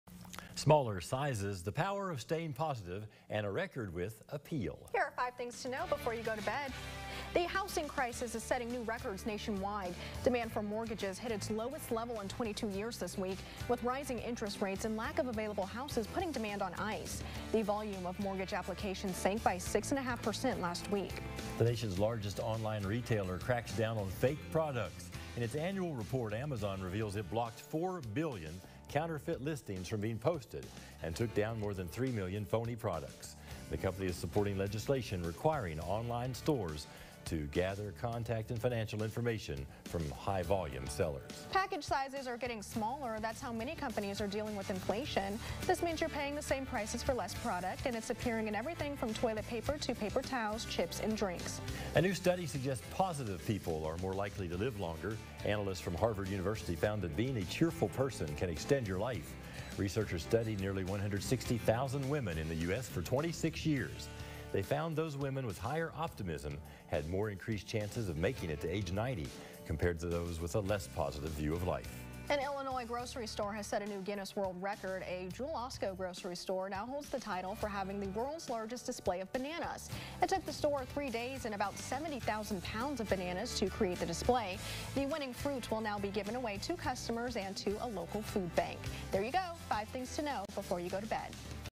June 9: Mortgages drop, shrinkflation, and being positive
Here are five things to know before you go to bed on June 9, 2022.